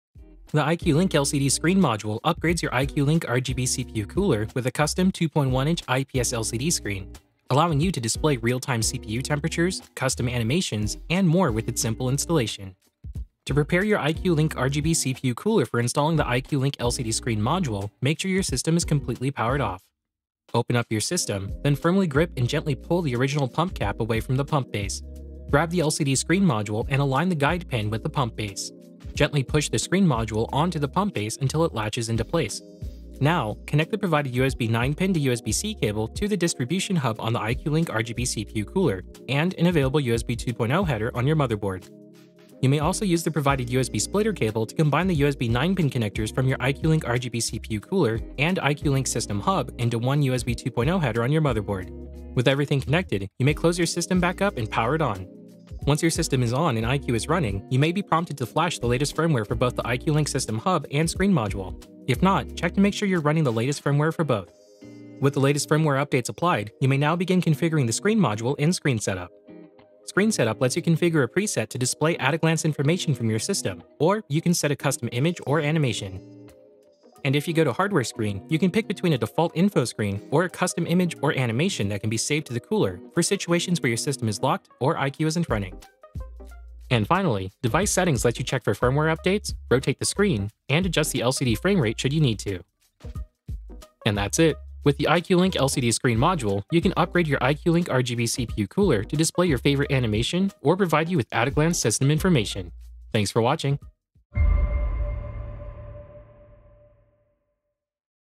How to Install the iCUE LINK AIO LCD Screen Module
The iCUE LINK LCD Screen Module upgrades your iCUE LINK RGB CPU Cooler with a custom 2.1” IPS LCD screen. Allowing you to display real-time CPU temperatures, custom animations, and more with its simple installation.